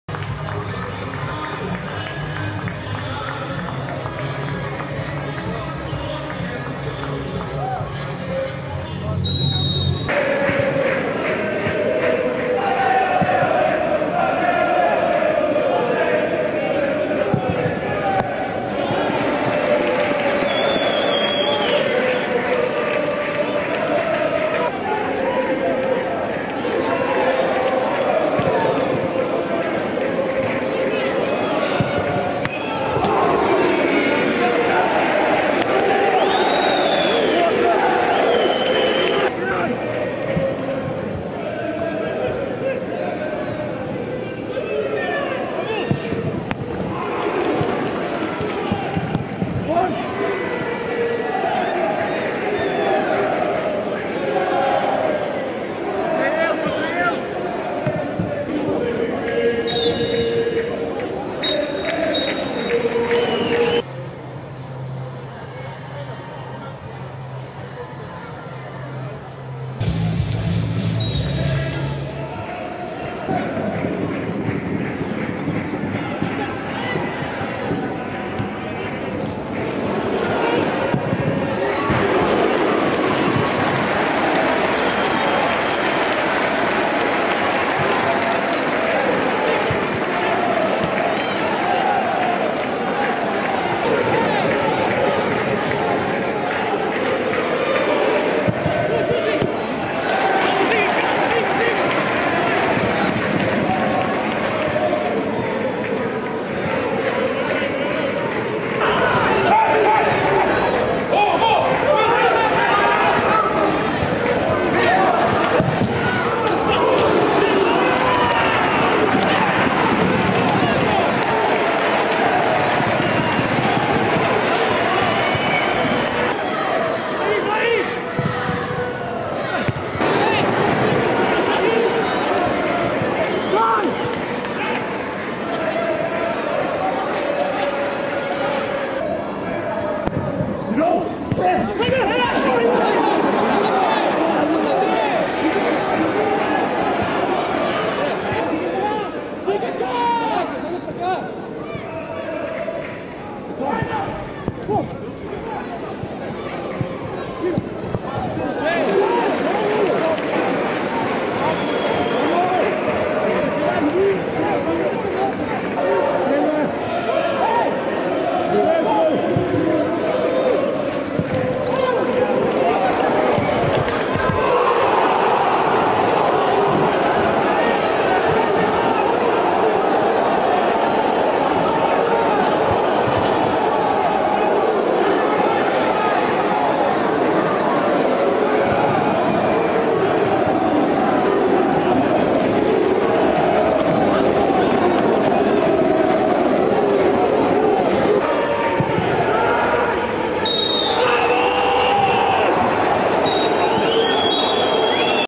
Highlights | Resumo: Paços de Ferreira 0-1 Gil Vicente (Liga 21/22 #14)
Golo de Leautey ao cair do pano, a dar a vitória ao Gil Vicente na casa do Paços de Ferreira, no jogo inaugural da 14.ª jornada da Liga Bwin.